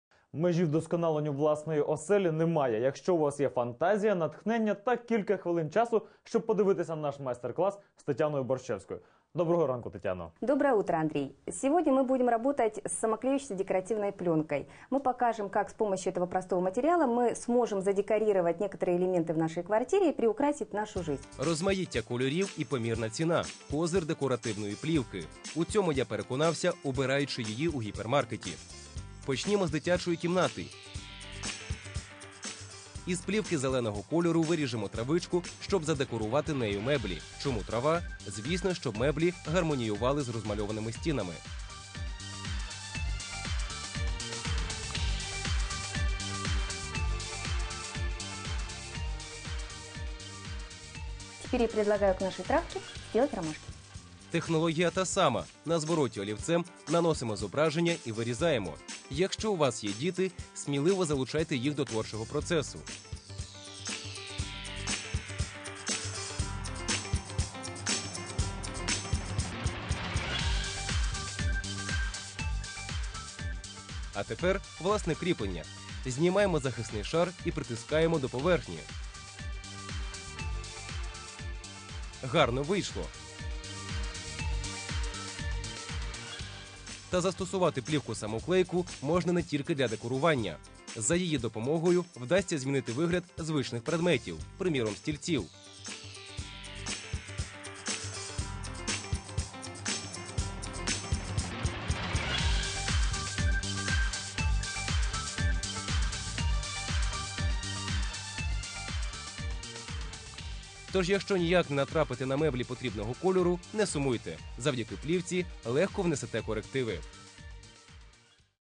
Декорируем мебель: Просто и со вкусом
ubr.ua - украинский бизнес ресурс : &#65279;&#65279;Предела совершенствованию собственного дома нет, когда есть фантазия, вдохновение и несколько минут времени, чтобы посмотреть мастер-класс от нашего дизайнера Татьяны Борщевской. .